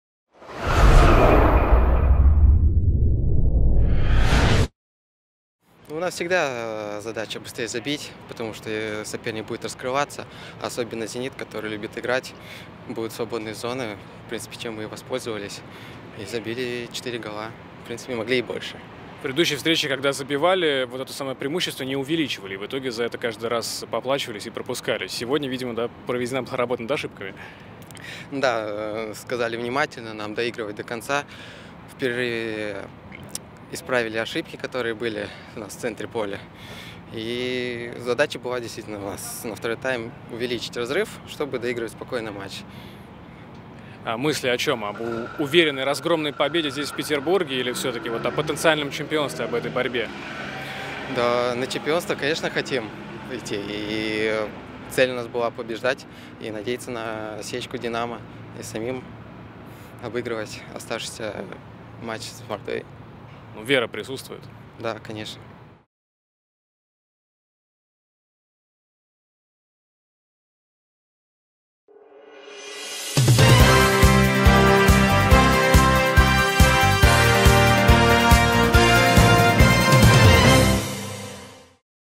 Анисимов: Могли забить «Зениту» больше
Нападающий «молодежки» Денис Анисимов рассказал о разгроме «Зенита» (4:1) в 29-м туре первенства России.